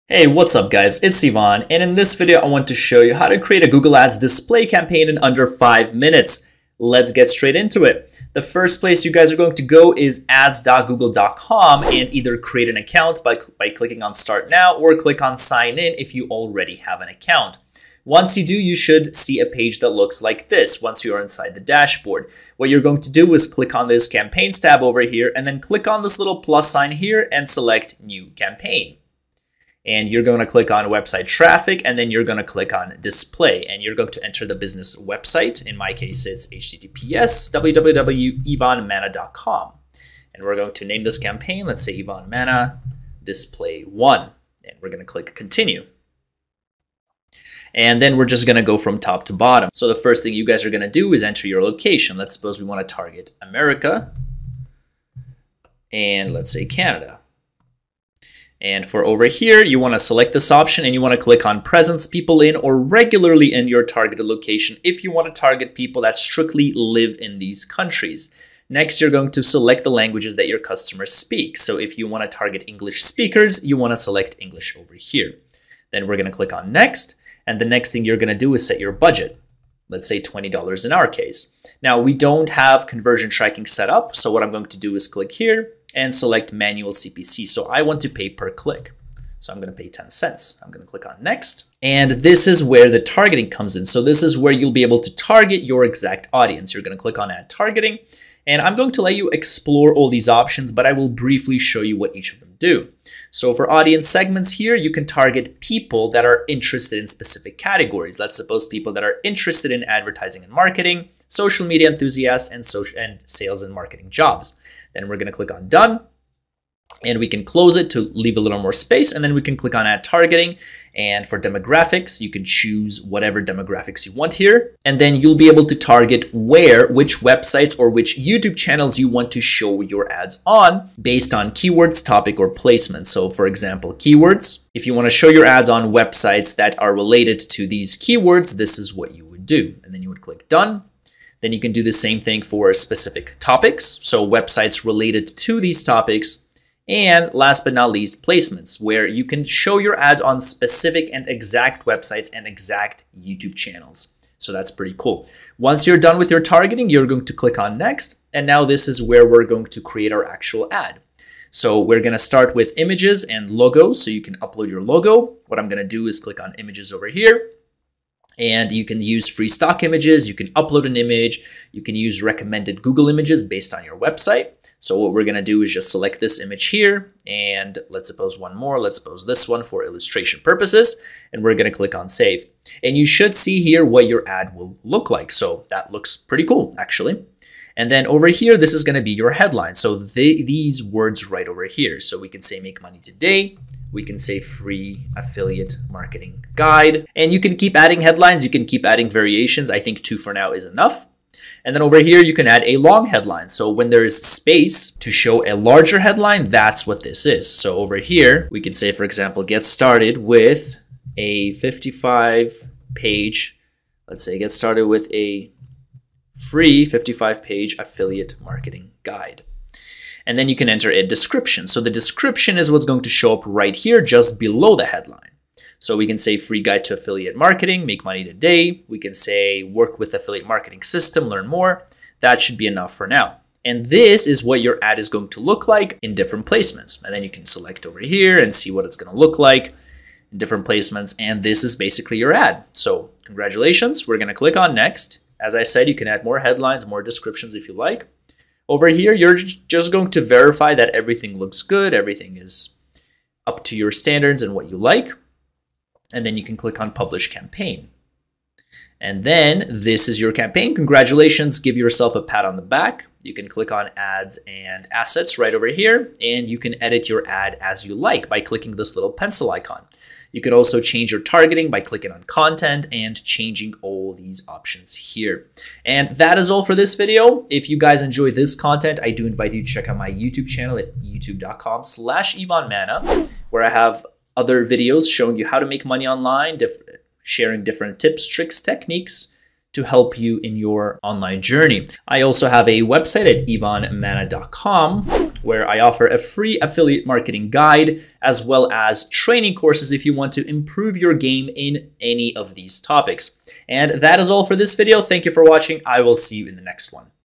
Google Display Ads Tutorial In UNDER 5 MINUTES | QUICKEST Tutorial on YouTube!
Don't have time but want to create a Google Ads display campaign?

I got you covered. In this tutorial, I'm going to show you how to create a Google Ads display campaign in JUST 5 MINUTES!

Follow along with me and follow the steps as I take you through the entire campaign-creation process from start to finish, going over everything you need to know to create your Google Ads campaign.